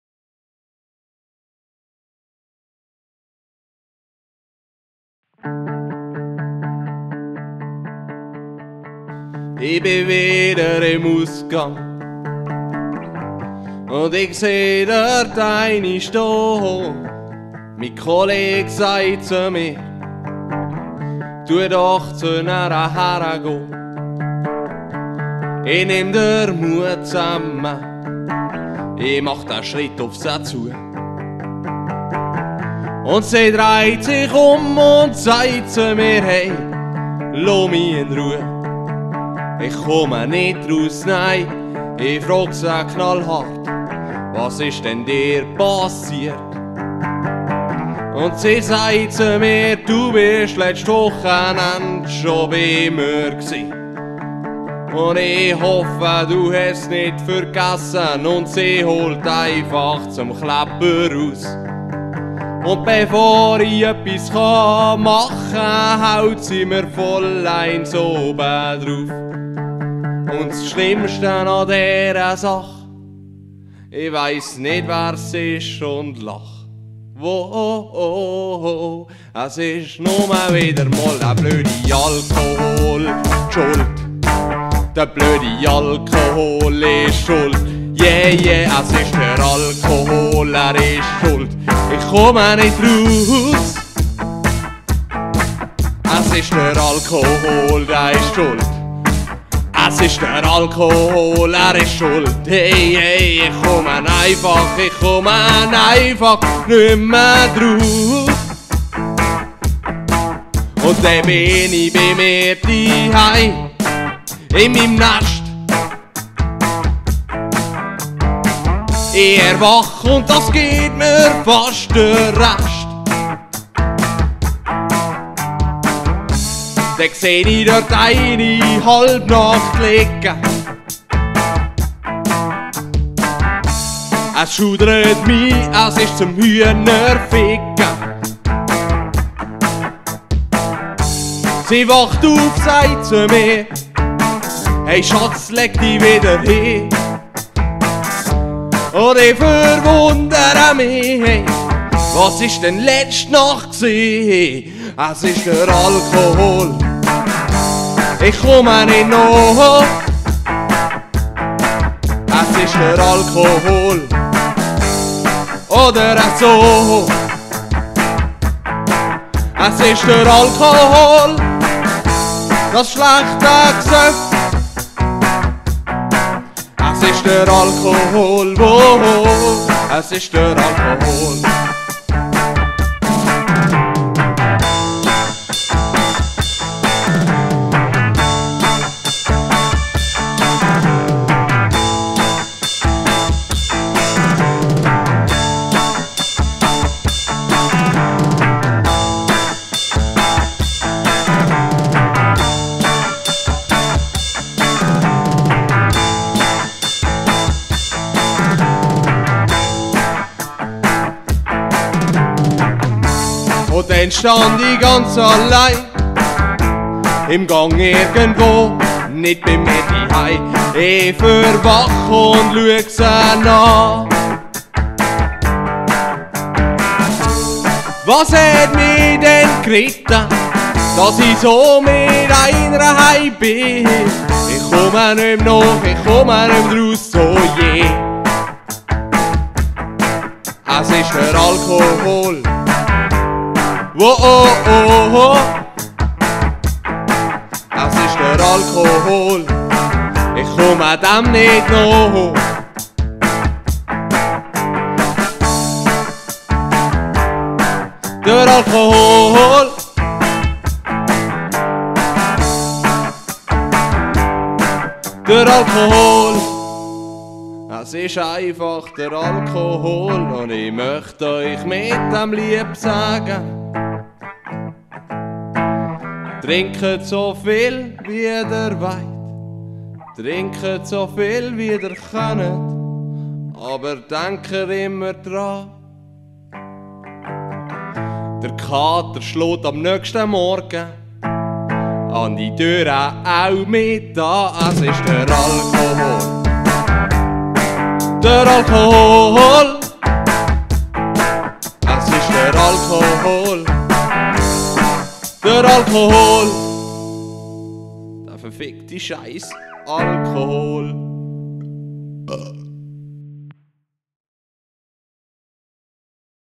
Alkohol
Jeder kennt es am Morgen nach dem Gelage im Ausgang. der Liebe Kater, die vergessene Erinnerung und die Freude wenn man bemerkt, dass man nicht alleine nachhause ging- unser Alkohol